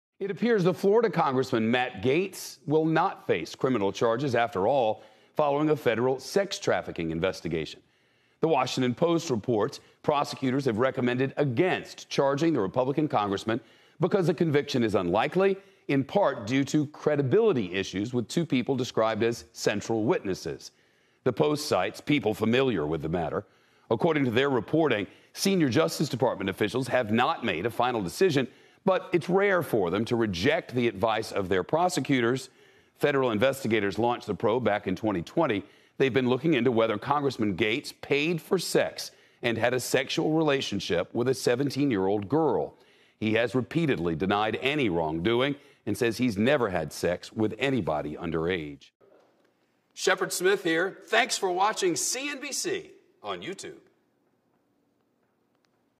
Florida Congressman Matt Gaetz unlikely to face charges in sex-trafficking probe
CNBC's Shep Smith reports that Rep. Matt Gaetz (R-Fla.) is not likely to be charged in a sex-trafficking probe.